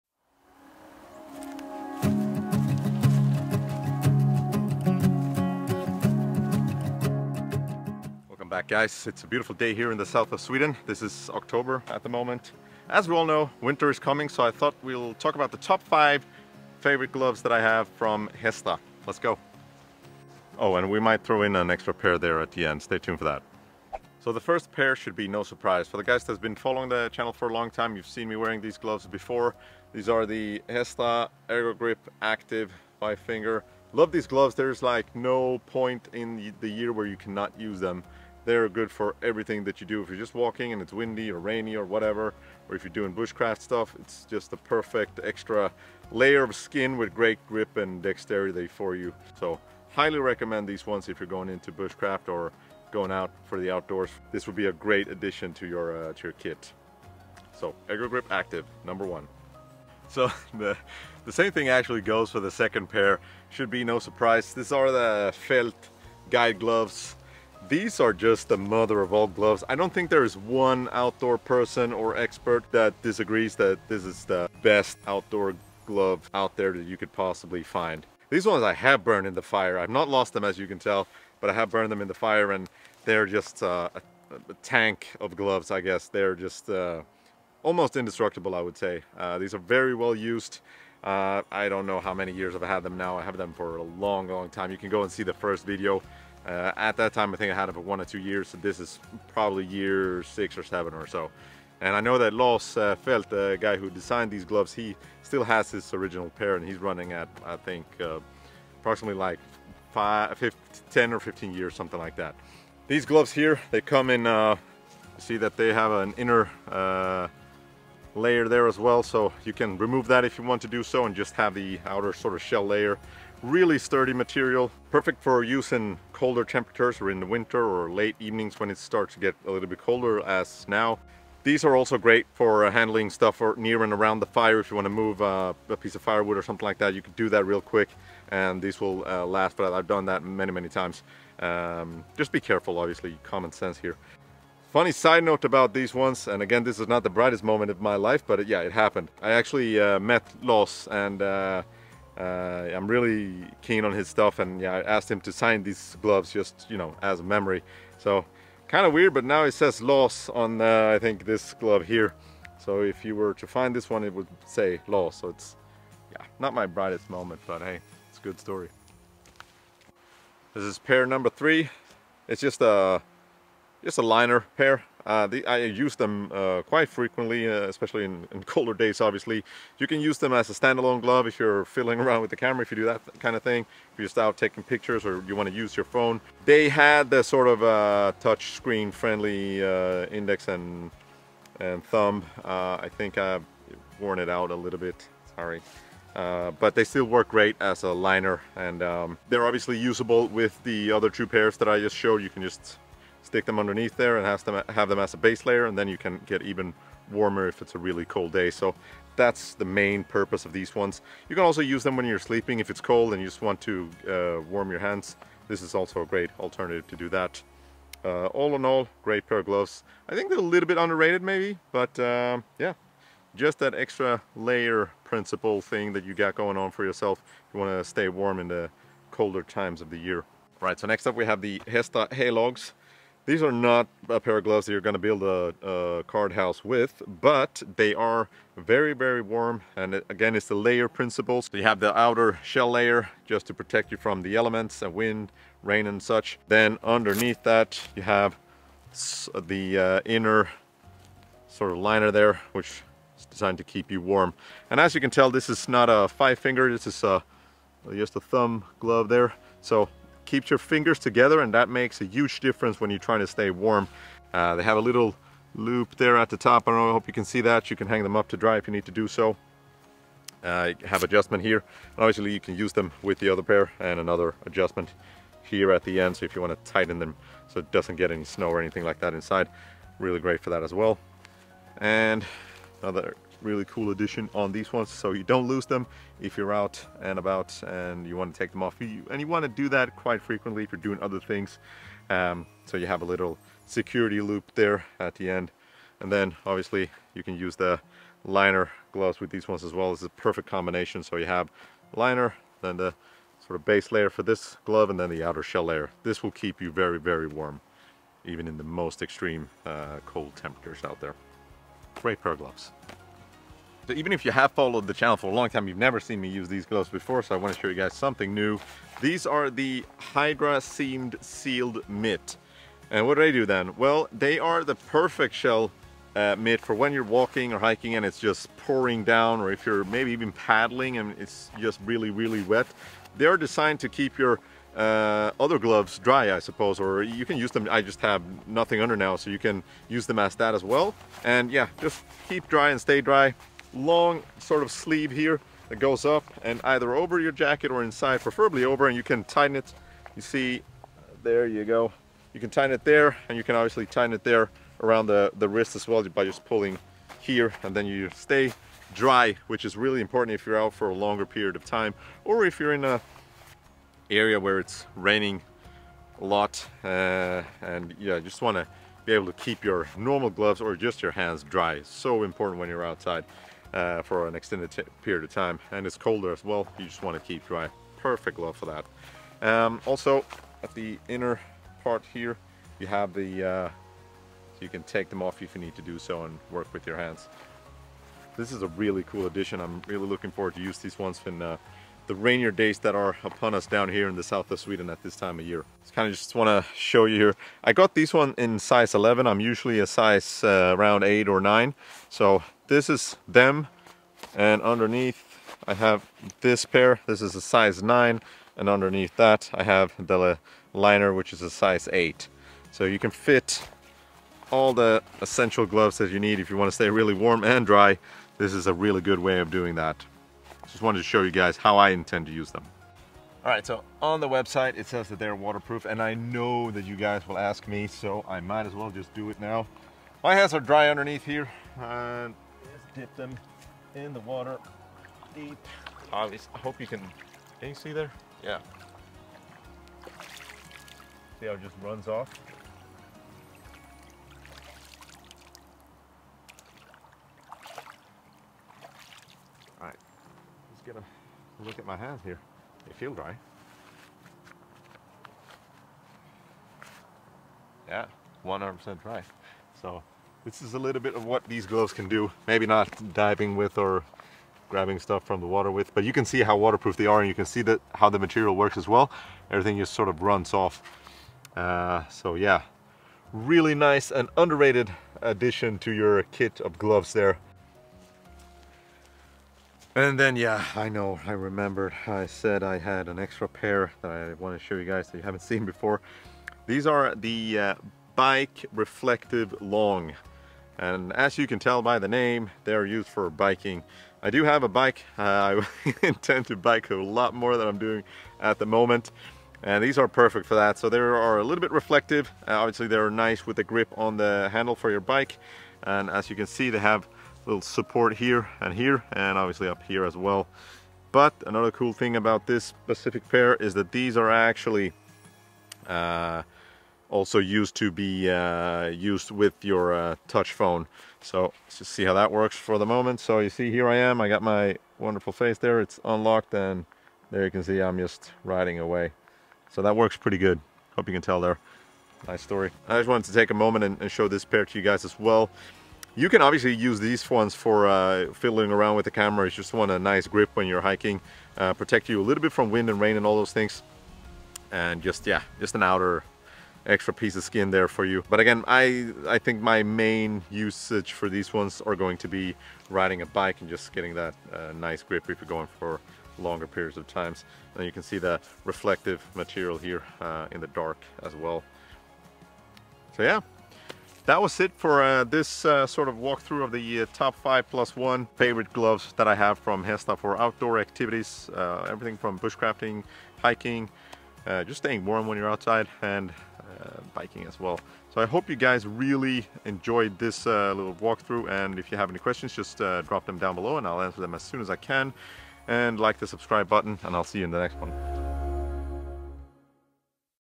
The Top 5 outdoor gloves from Hestra!
Since 1936 Hestra has been making some of the best gloves out there, here we take a closer look at my top 5 favorite gloves for the great outdoors, both in winter and summer!

The drone I use
Main camera
24 MM lens